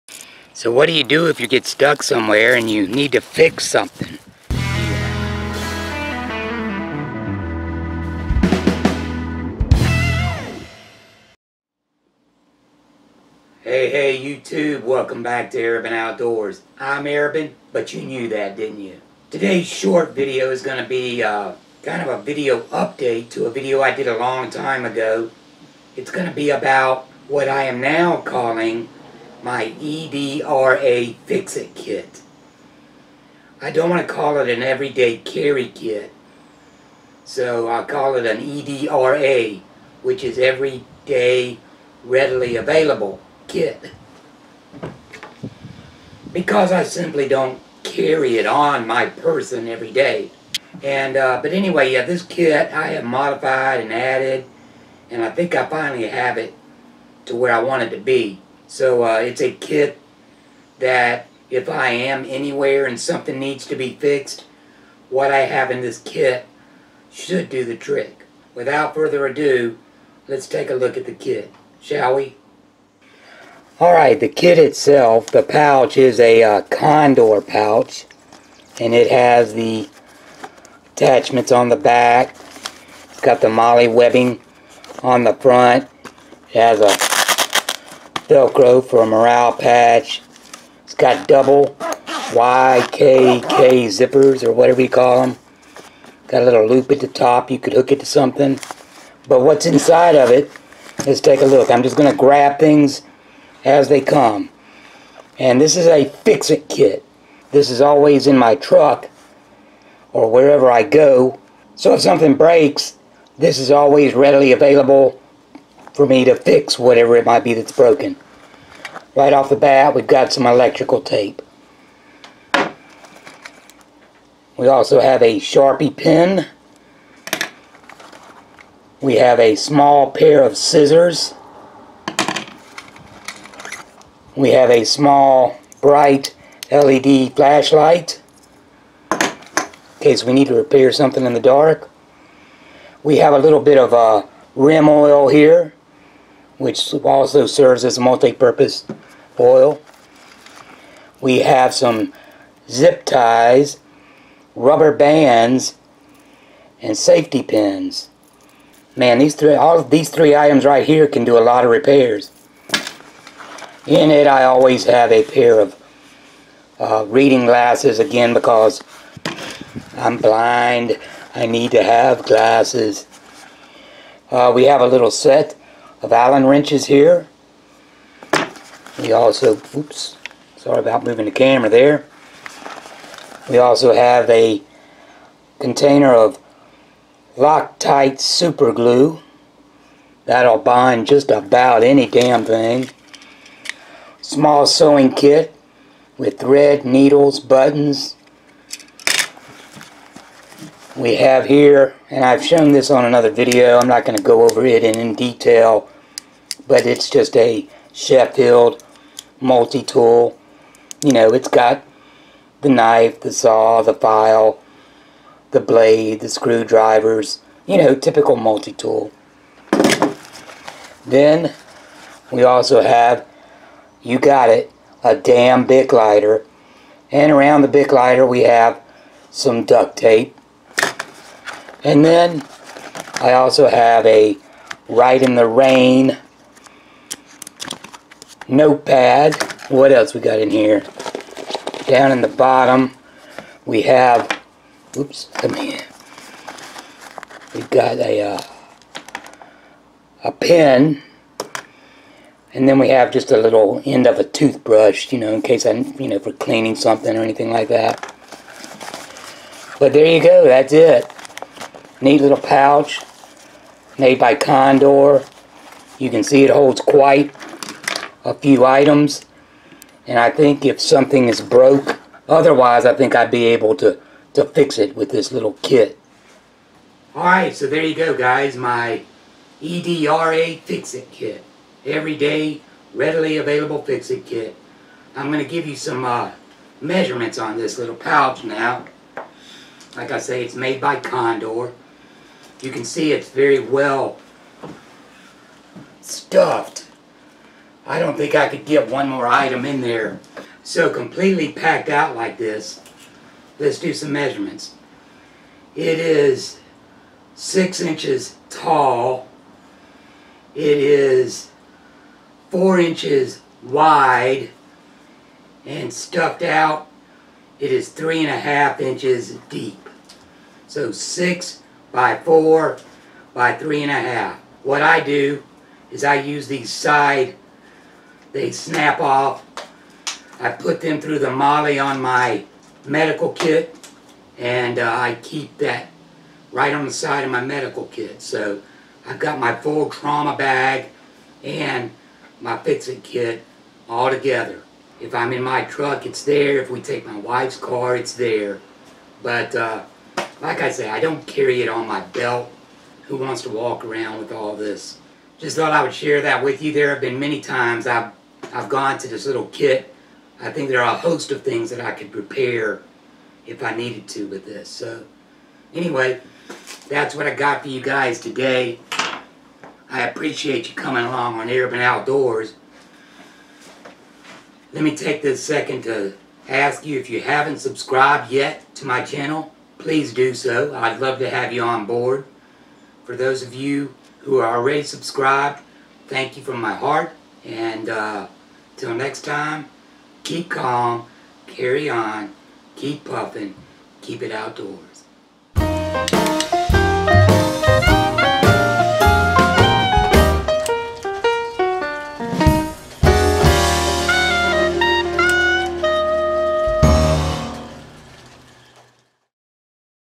EDRA Fix it Kit
Update on my Condor Every Day Readily Available Fix It Kit.
Not EDC cause I don't actually carry it on my person every day. But it is always in whichever vehicle I am driving at the time. Therefore, readily Available.